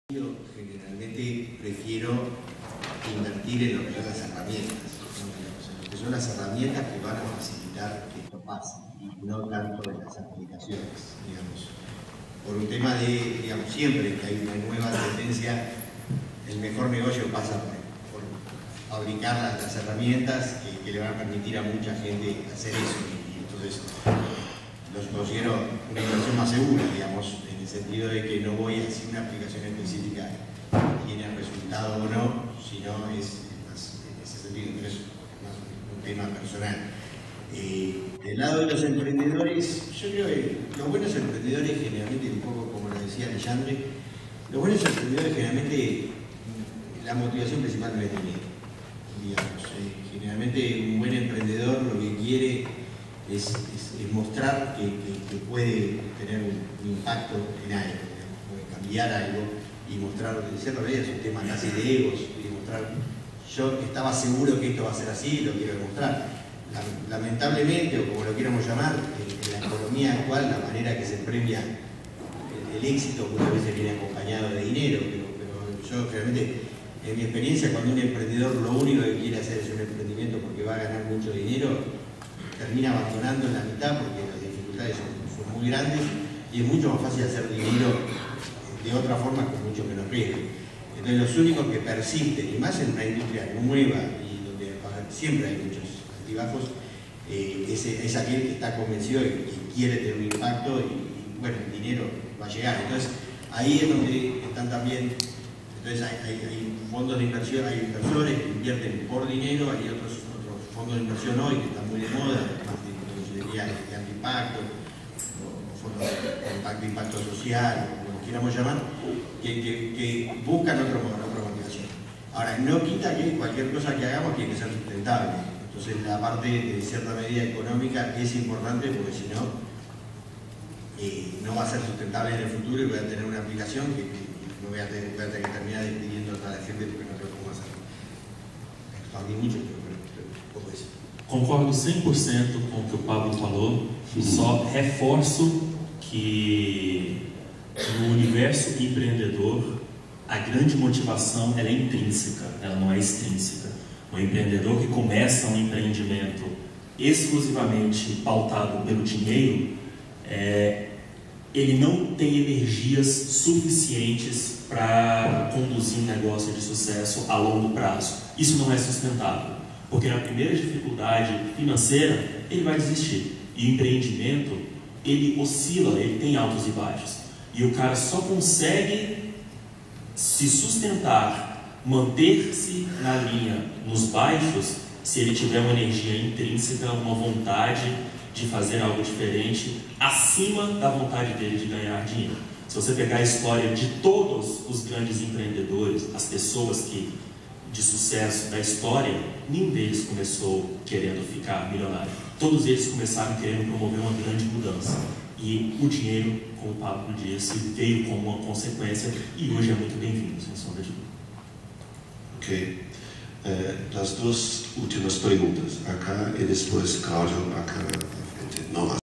Pablo Brenner y Alexandre Gomes sobre emprendedores
Pablo Brenner, de VP Innovation Consulting y Alexandre Gomes de Seastecnología, hablando sobre emprendedores. Mesa redonda: Datos abiertos y la generación de modelos de negocio sustentables. Conferencia Regional de Datos Abiertos para América Latina y el Caribe. Hotel Radisson, Montevideo, Uruguay, jueves, ‎27‎ de ‎junio‎ de ‎2013, 10:05am.